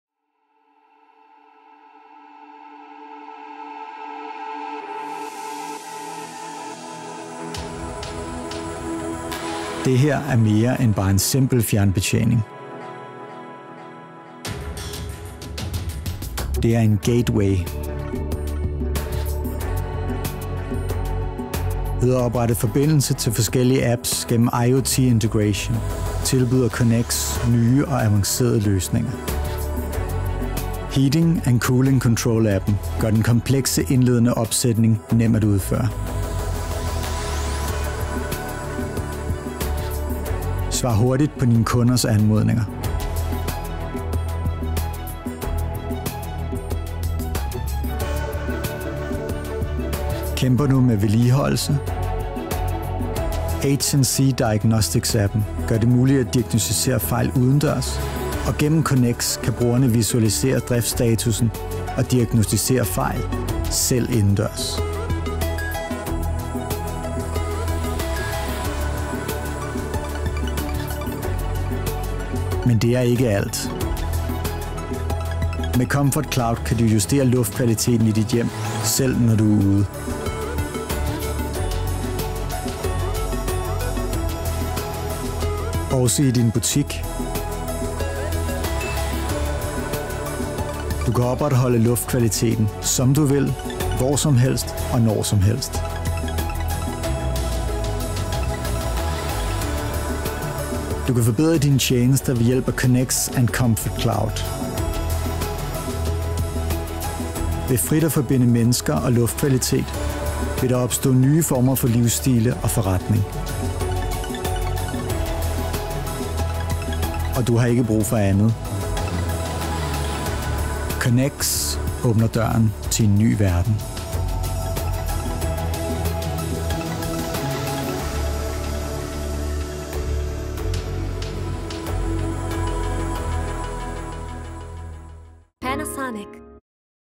DA – CONEX Controllere og apps IOT integreret
CONEX – Dette er mere end blot en simpel fjernbetjening. CONEX kombinerer controllere og apps gennem IoT-integration. Med disse fleksible kontrolmuligheder er du nu i stand til at få nem adgang til fjernbetjening via Wi-Fi, værktøjer til diagnose og fejlfinding, detaljerede betjenings- og vedligeholdelsesindstillinger. Oplev vores nye løsning!

00:09 Intro
00:10 Ud over den enkle fjernbetjening
00:20 Apps
00:30 H&C Control App
00:49 H&C Diagnosis App
01:12 Comfort Cloud App
01:49 Forbinde r mennesker frit